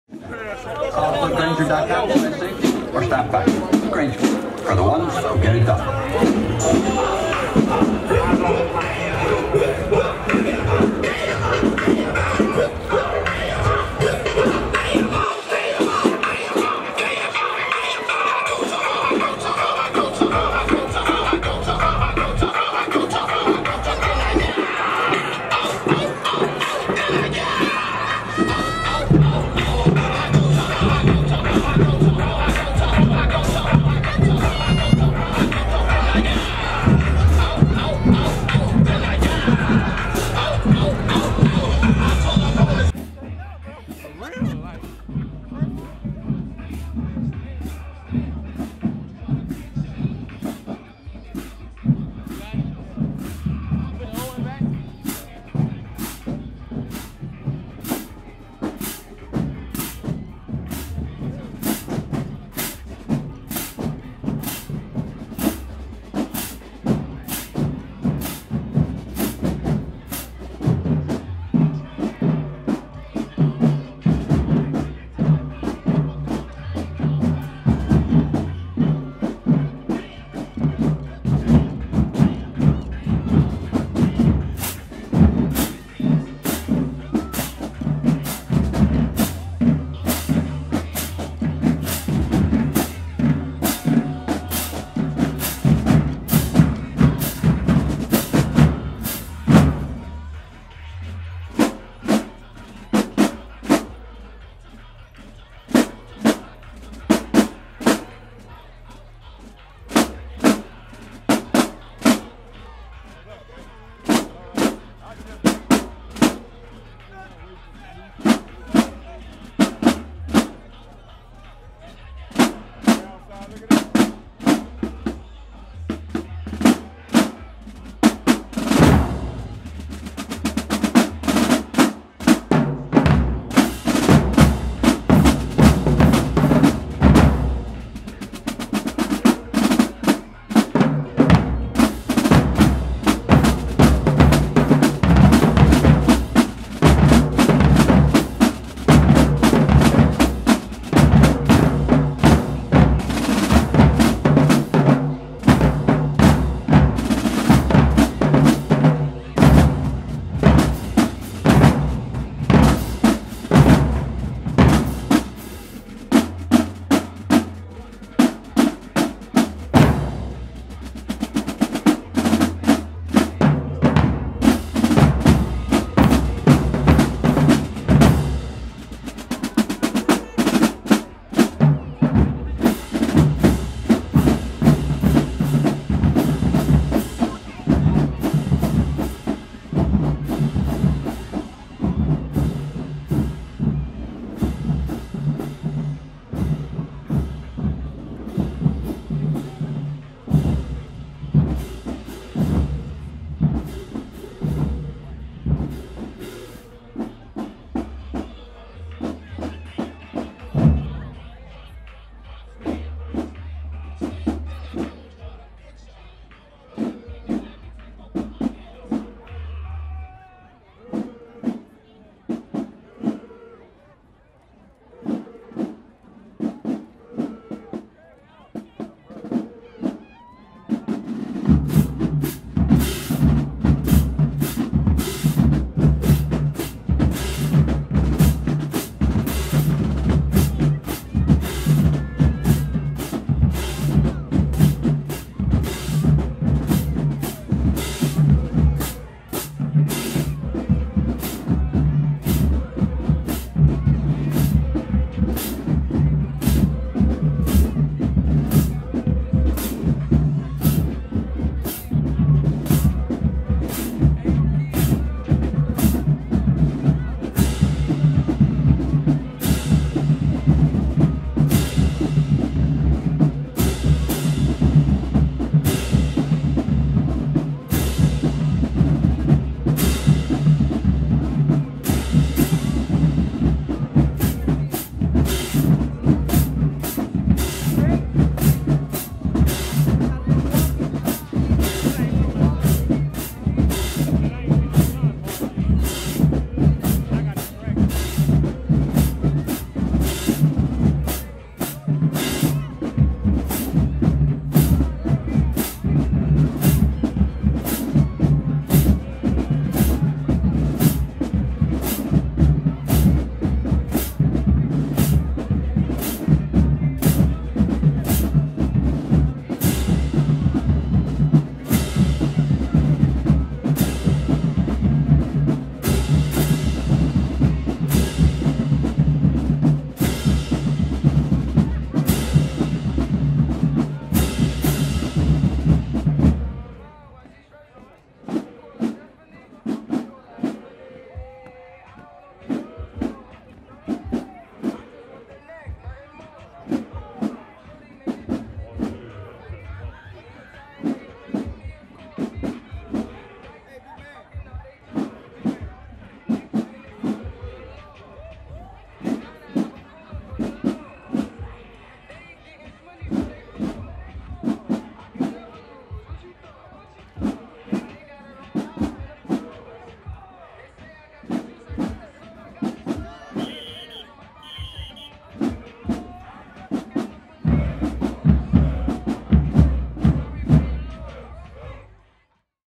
Southern University Human Jukebox 2016 Marching In | Homecoming
My Favorite Video so far this year.
Homecoming 2016
Southern University Human Jukebox
Be Sure To Watch in HD! ||| @DaEdge1 | Human Jukebox Media

Human Jukebox Media Videographers/Photographers: Garrett Edgerson, Perry White, Zachary Daigle, Chris Barber, Darby Smith, Derrick Espree